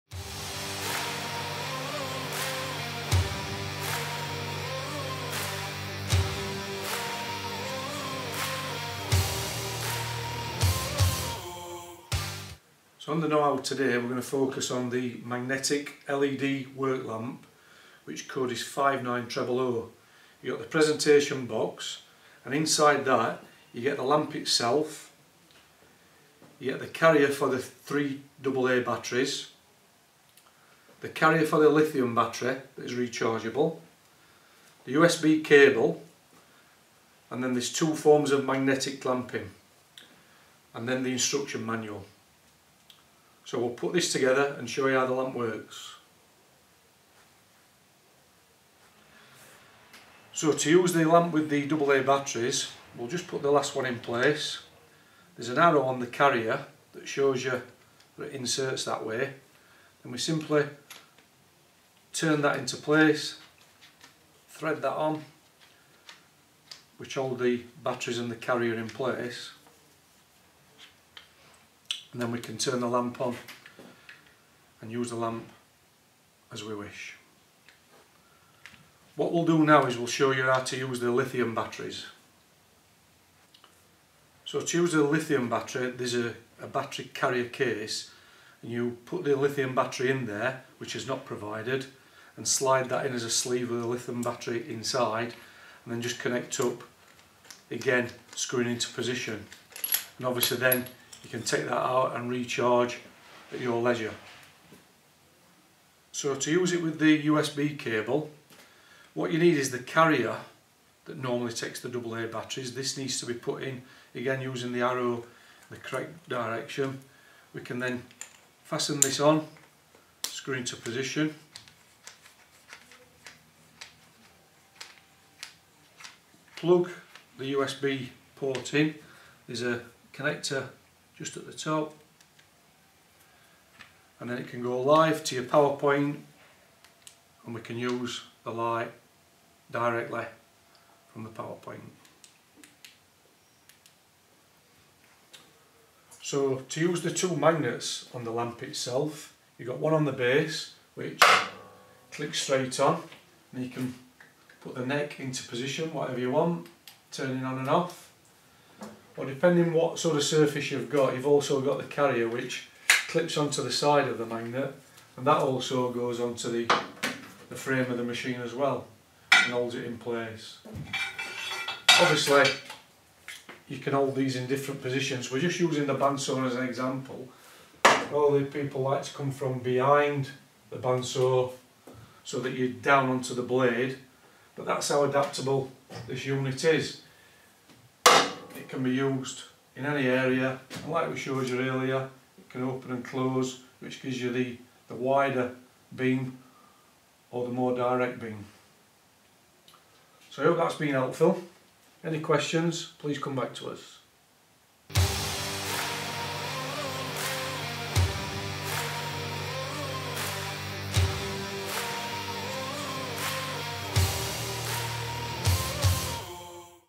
Know-How Series: 59000 Magnetic LED Work Lamp
This week Craig Heffren provides an overview of the 59000 Magnetic LED Work Lamp (59000)